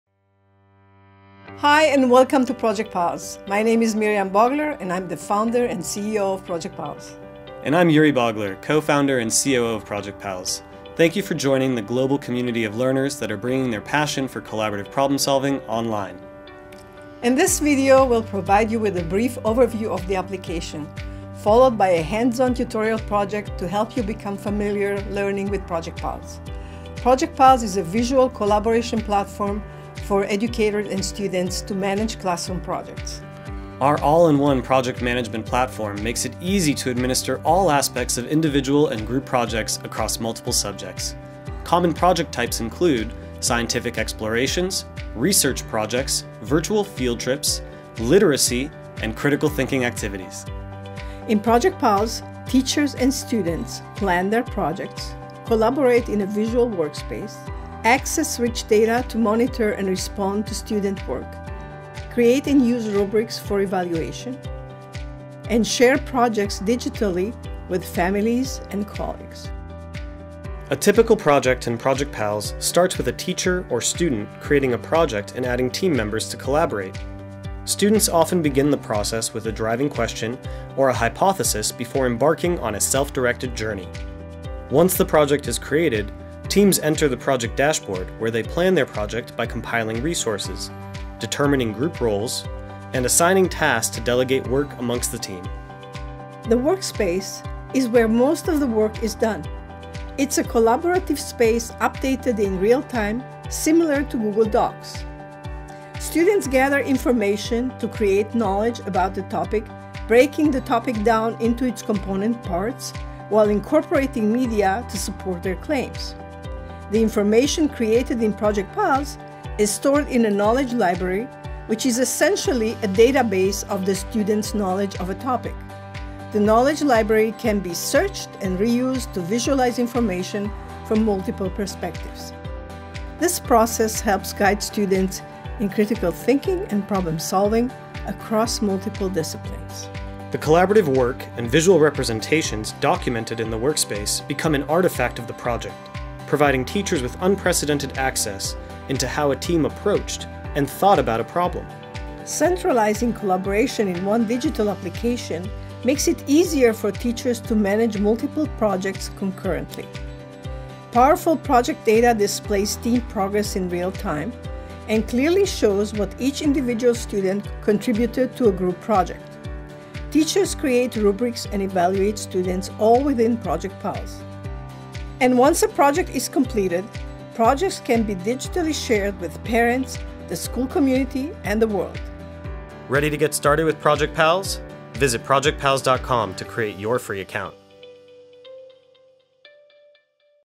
Project Pals platform overview
Watch a brief 3 minute overview video about Project Pals' visual collaboration platform for project based learning. Join progressive educations worldwide who are using Project Pals to manage classroom projects.